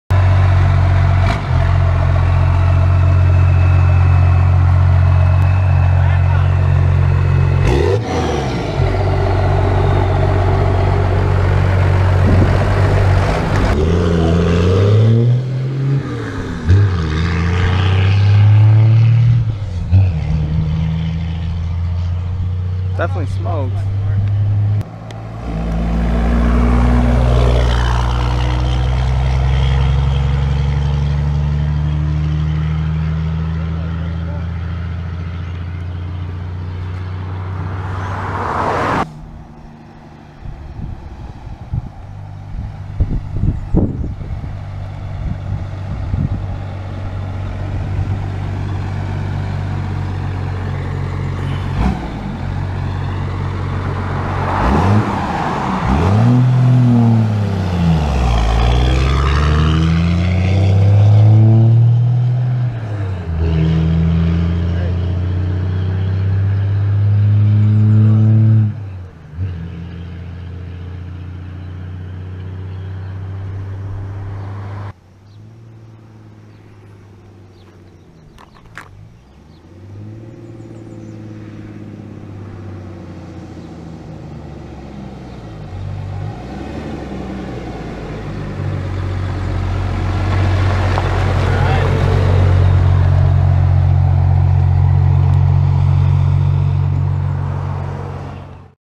1st Gen Cummins: DRIVE-BY
1989 D250 12 Valve Cummins with the 5-speed Getrag.

First test drive with the custom fuel pin. (Yes the truck does have a hood)

MODS INCLUDE:
Custom 5" straight pipe into 8" mitre-cut stack
Custom DIY intake
Custom fuel pin (similar to Denny T)